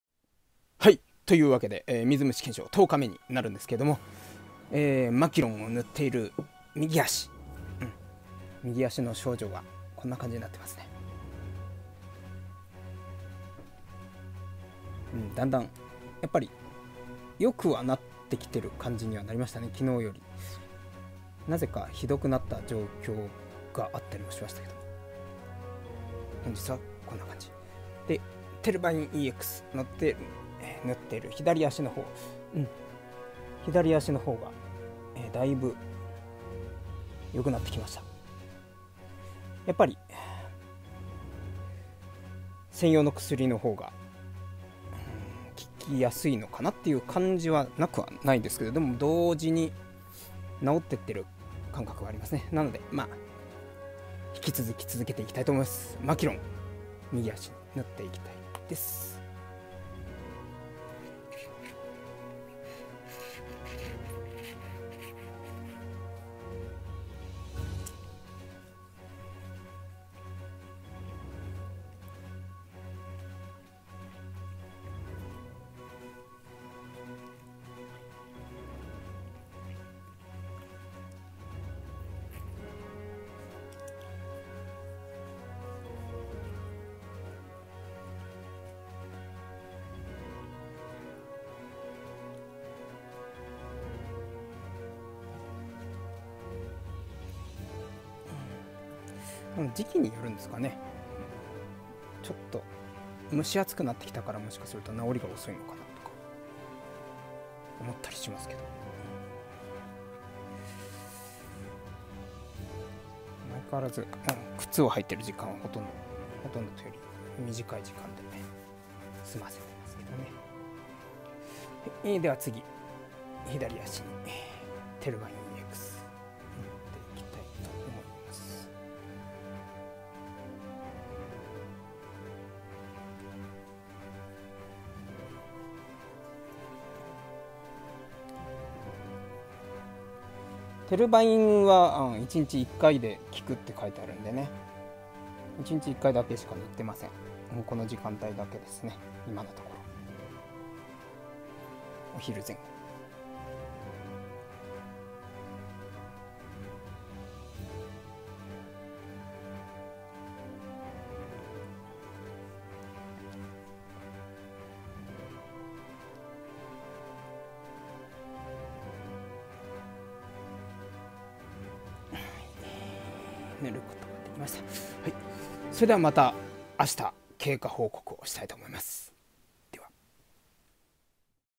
【水虫検証1-10】10日目、かなり回復した感じが見えてきました。
昨日の状態からだと少し不安を感じましたが。。
今朝になったら、見た目も感覚もかなり良くなった感を受けました！